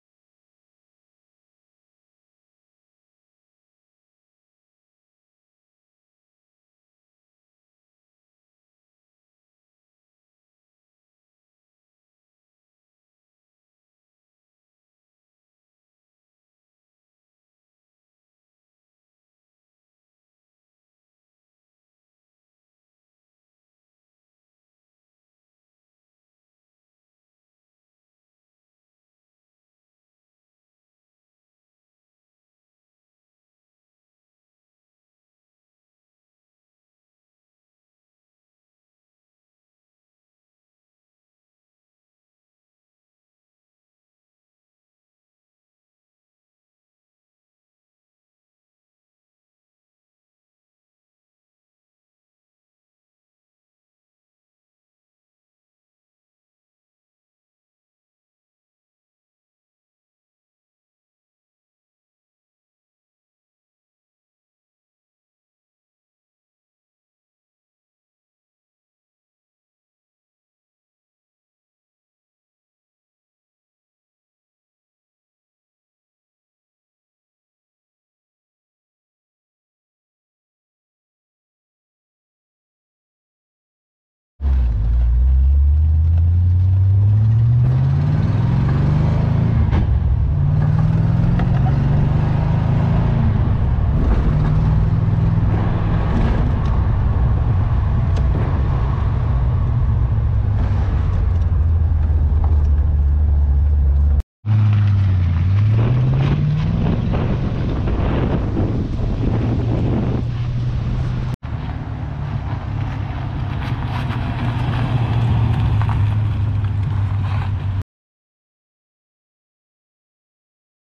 1994 MG RV8
Anglia Car Auctions Kings Lynn are pleased to offer this 1994 MG RV8 as an entry in our November 6th and 7th classic car auction.